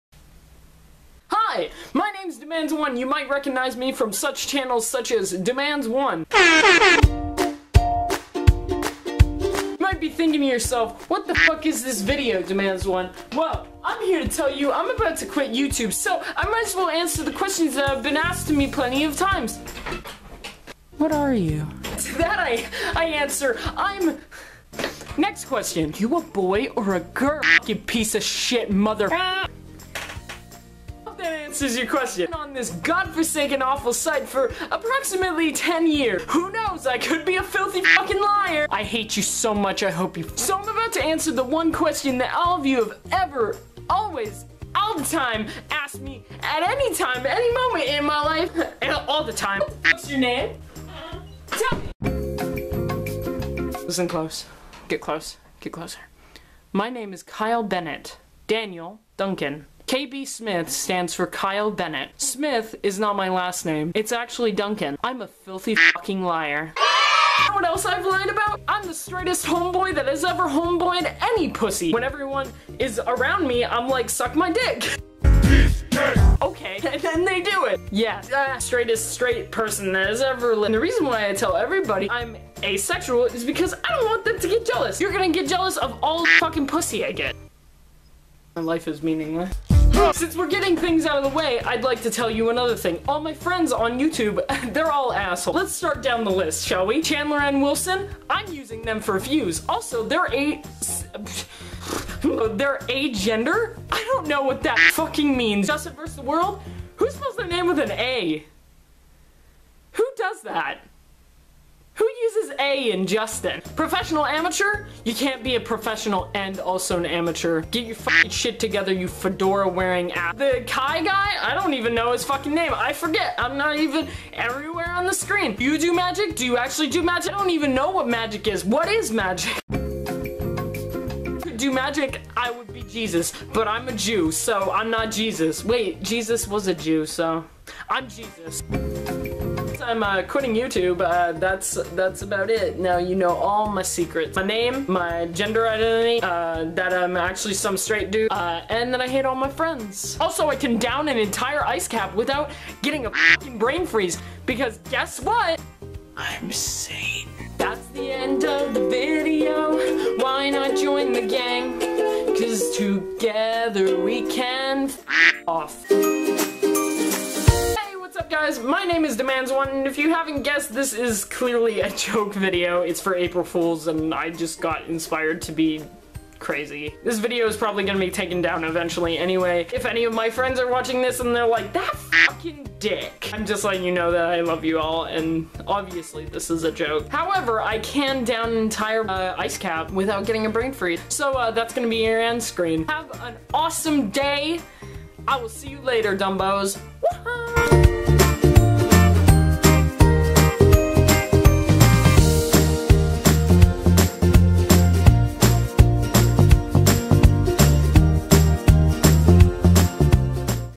Quiting Youtube (April Fools)
I'm sorry, it's too much, so let me get some stuff off my chest for you guys